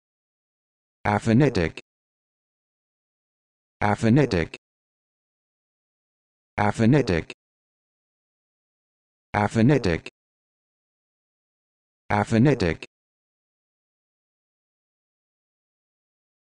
How to Pronounce Aphanitic
This video shows you how to pronounce Aphanitic